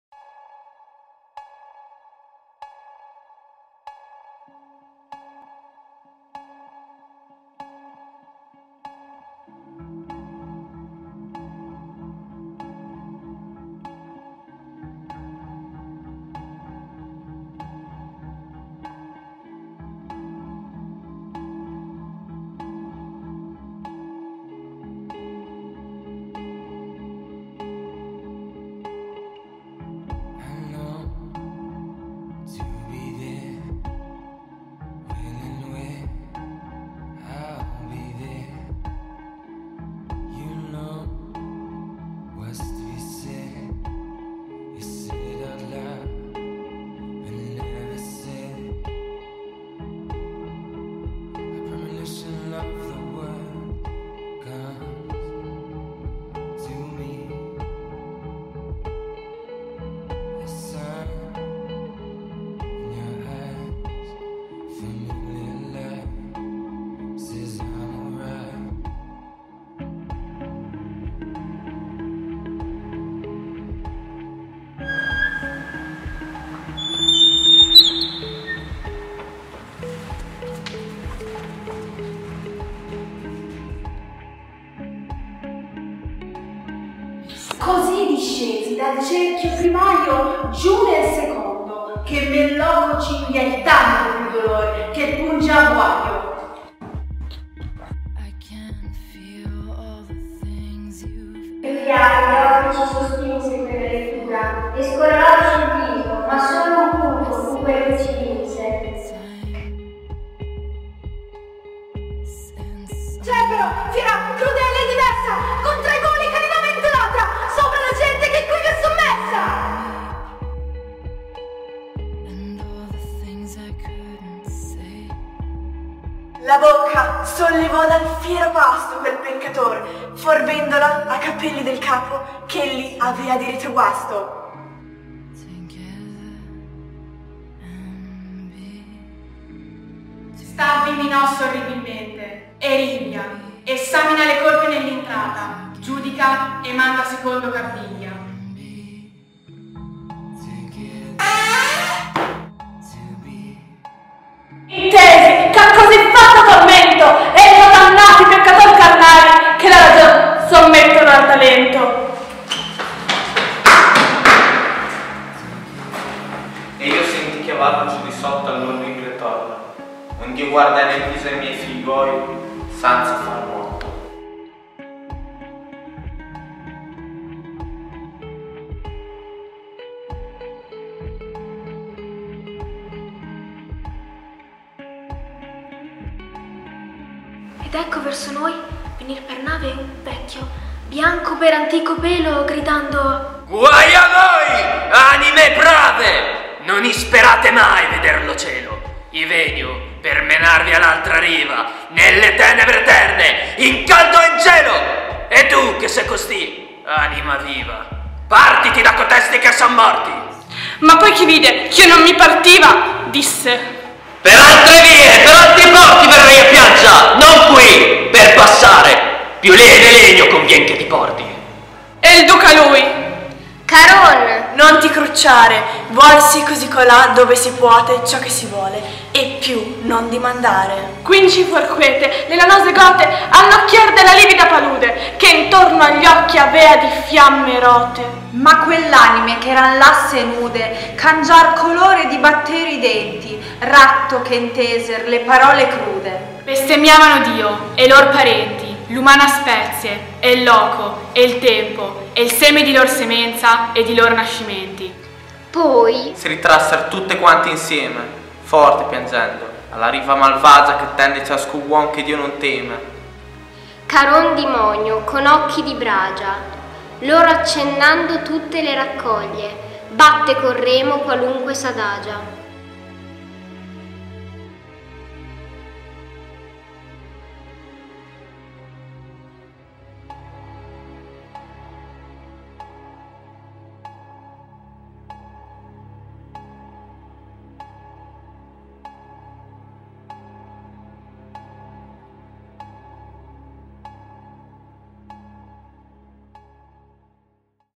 Traghettatori di anime
Video presentato all'11° Festival Dantesco dai ragazzi della classe 3°T del Liceo Manfredo Fanti di Carpi. Video realizzato in collaborazione con i Musei e il Teatro Comunale.